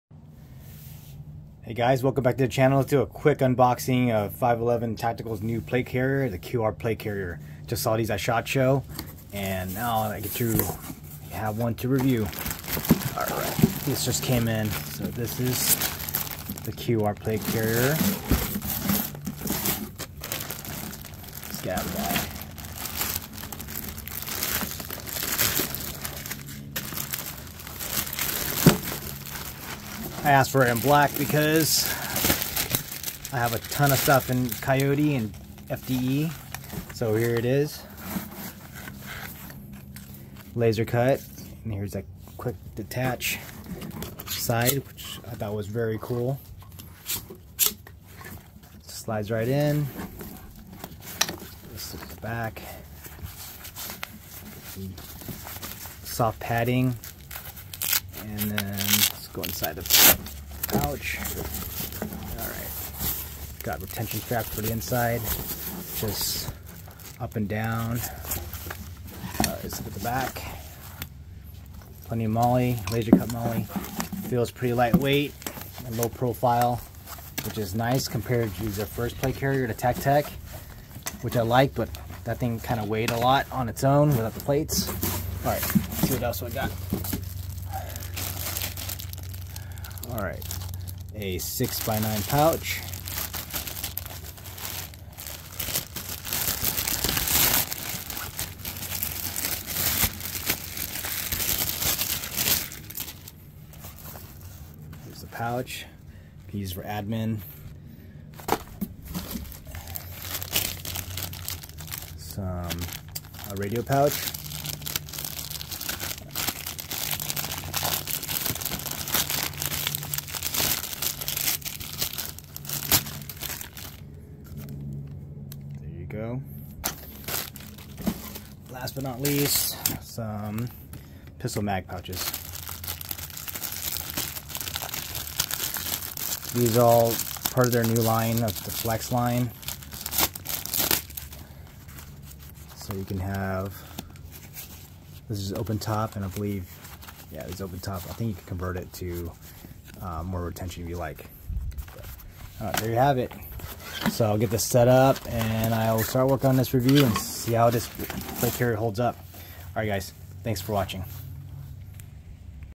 Unboxing 5.11 Tactical QR Plate Carrier
Just got in 5.11 Tactical's QR Plate Carrier!